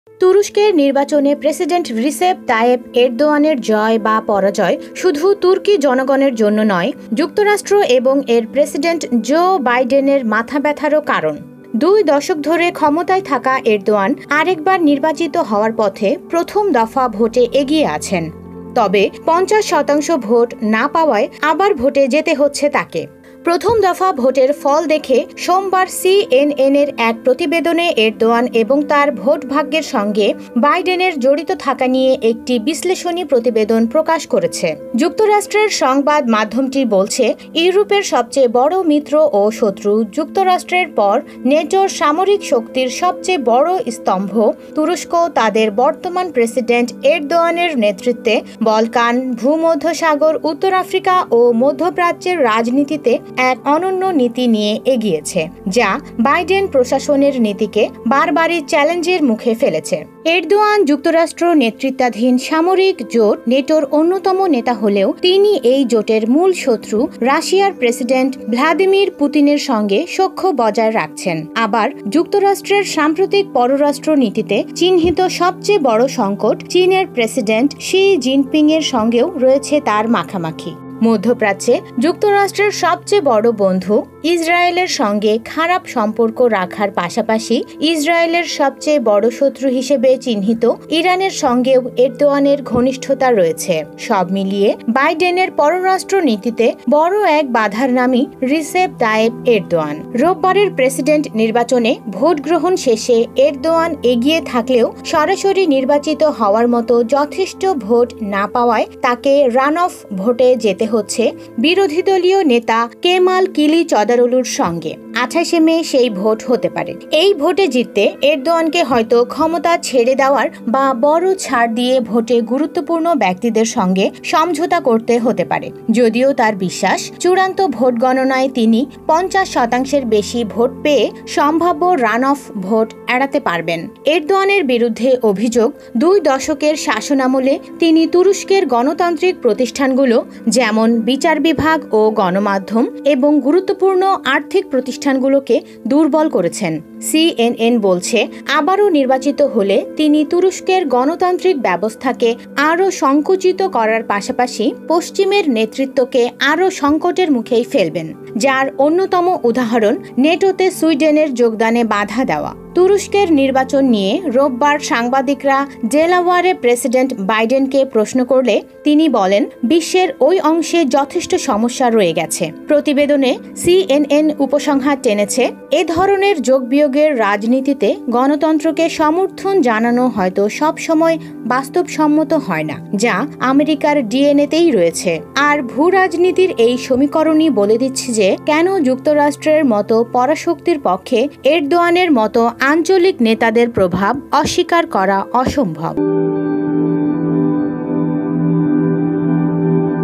এরদোয়ানের ভোট নিয়ে কেন বাইডেনের এতো মাথাব্যথা ? পশ্চিমাদের কথা শুনেন না এরদোয়ান ?
তুরস্কের নির্বাচনে প্রেসিডেন্ট রিসেপ তাইপ এরদোয়ানের জয় বা পরাজয় শুধু তুর্কি জনগণের জন্য নয়, যুক্তরাষ্ট্র এবং এর প্রেসিডেন্ট জো বাইডেনের মাথাব্যথারও কারণ।

দু্ই দশক ধরে ক্ষমতায় থাকা এরদোয়ান আরেকবার নির্বাচিত হওয়ার পথে প্রথম দফা ভোটে এগিয়ে আছেন। তবে ৫০ শতাংশ ভোট না পাওয়ায় আবার ভোটে যেতে হচ্ছে তাকে।

প্রথম দফা ভোটের ফল দেখে সোমবার সিএনএনের এক প্রতিবেদনে এরদোয়ান এবং তার ভোট ভাগ্যের সঙ্গে বাইডেনের জড়িত থাকা নিয়ে একটি বিশ্লেষণী প্রতিবেদন প্রকাশ করেছে।

যুক্তরাষ্ট্রের সংবাদ মাধ্যমটির বলছে ,  ইউরোপের সবচেয়ে বড় মিত্র ও শত্রু, যুক্তরাষ্ট্রের পর নেটোর সামরিক শক্তির সবচেয়ে বড় স্তম্ভ তুরস্ক তাদের বর্তমান প্রেসিডেন্ট এরদোয়ানের নেতৃত্বে বলকান, ভূমধ্যসাগর, উত্তর আফ্রিকা ও মধ্যপ্রাচ্যের রাজনীতিতে এক অনন্য নীতি নিয়ে এগিয়েছে, যা বাইডেন প্রশাসনের নীতিকে বার বারই চ্যালেঞ্জের মুখে ফেলেছে।